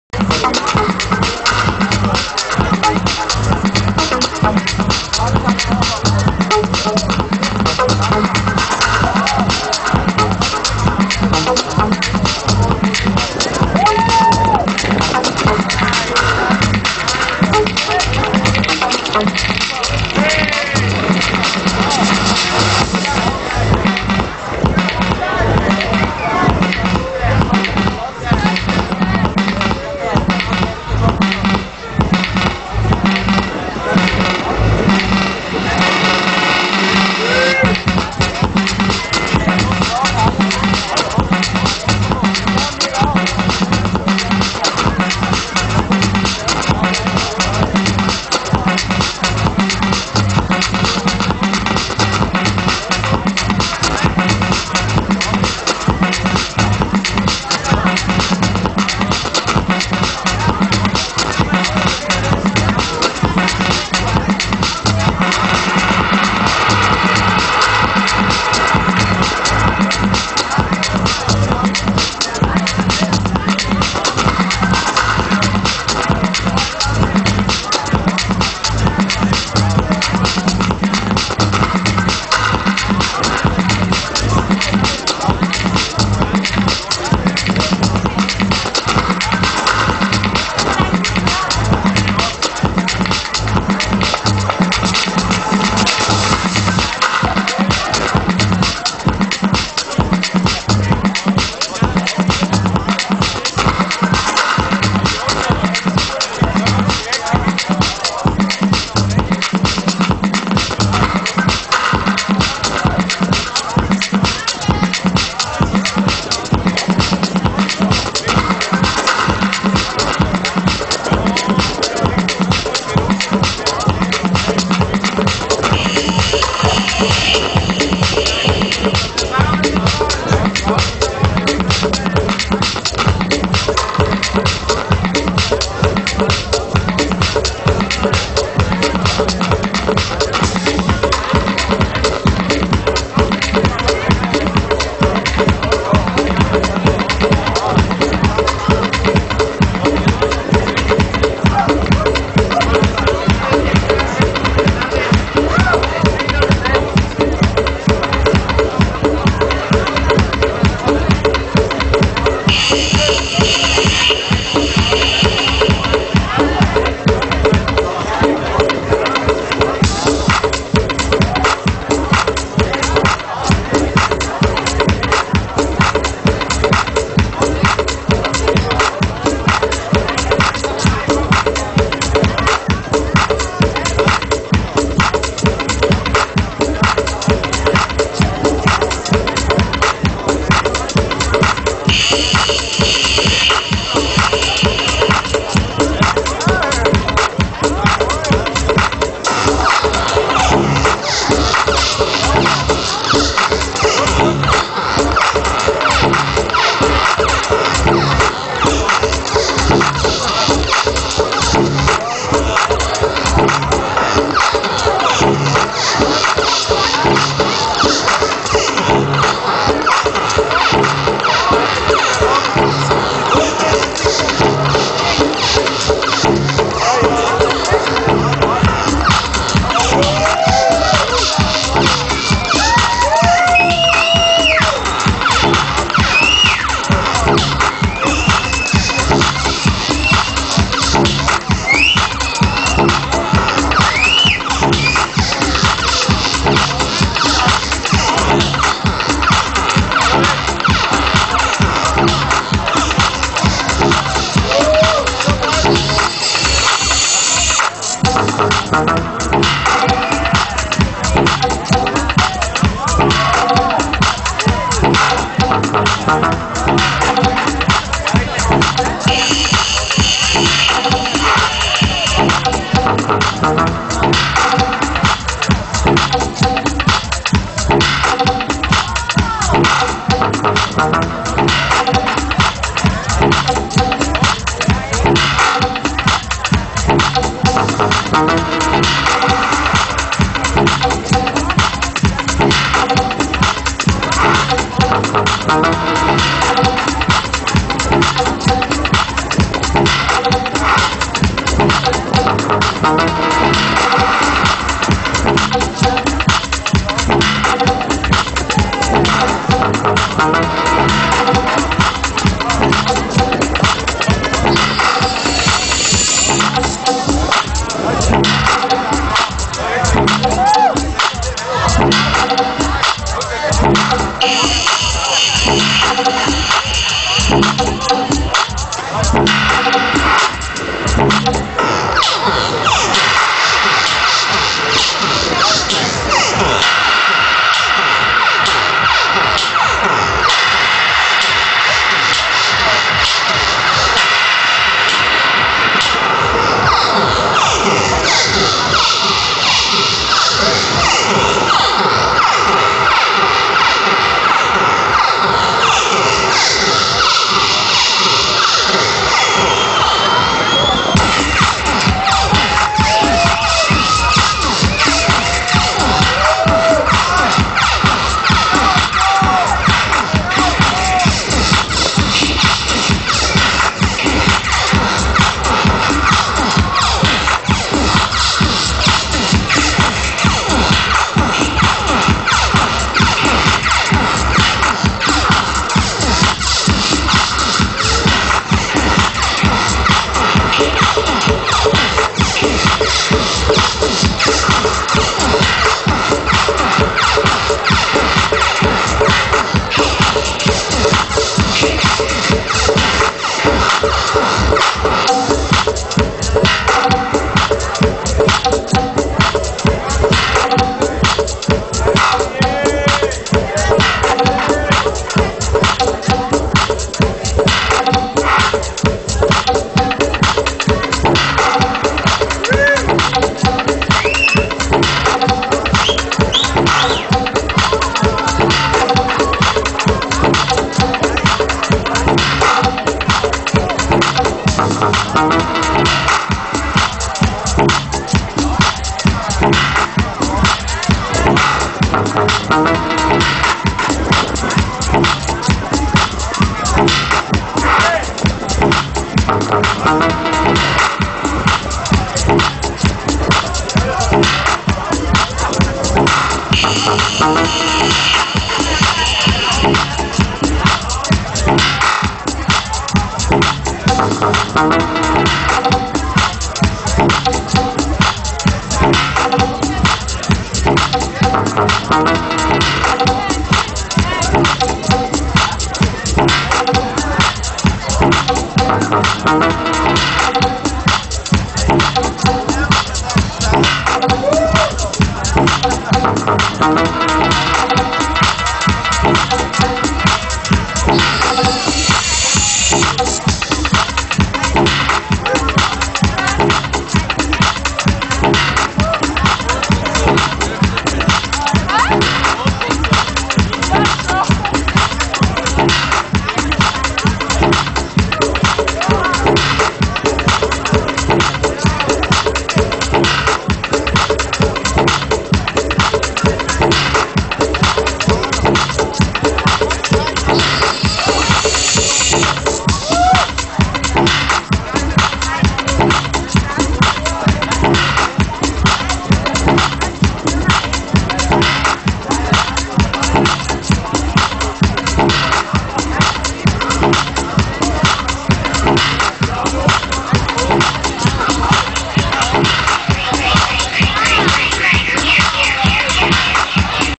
12/101 The Multicolor Pointer Sattelegg 2008
The Multicolor Pointer Final Edition Openair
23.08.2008 Start: 21:00 Uhr till 18:00 Uhr

DJ Tuned@work

LineUp:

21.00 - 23:00  Brothers Tunes LIVE
23:00 - 01:00  DJ Tuned
01:00 - 02:00  Orgom Flow LIVE
02:00 - 03:00  Dancing Devil LIVE
03:00 - 06:00  DJ`s Müstik & Fidimohnius
06:00 - 07:00  Motion Drive LIVE
07:00 - 09:00  DJ Idefix
09:00 - 10:00  Suncollector LIVE
10:00 - 12:00  DJ Pow-Low
12:00 - 13:00  Full Contact LIVE
13:00 - 14:00  Strandhaus LIVE
14:00 - 18:00  DJ`s Bocuse & To.B.